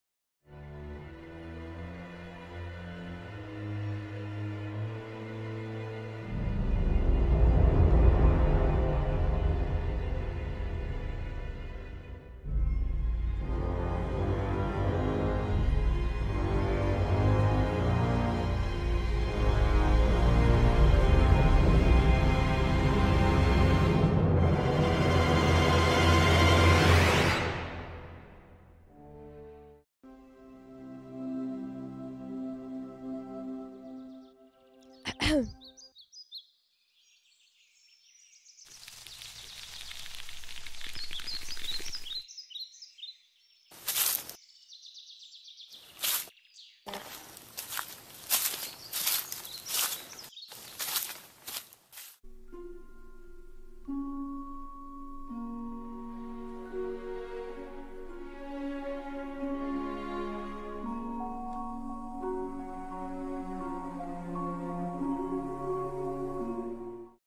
Through The Woods (STOP MOTION)
I made this for a class, and the assignment was to pretty much create anything we wanted as long as it took 9 weeks! And I have always wanted to make a stop motion something so I decided to create a stop motion animated trailer :)  It is fake just to clear that up… I know it doesn't look very professional but just in case :-) I am sorry it's so choppy, I really tried to create smooth movements!
But I very much hope you enjoyed it anyway!! :D

CHARACTERS
boy: (i haven't decided on one yet!) Wilbur, Gremly, Charlie
leaf: LEAF
rock: ROCK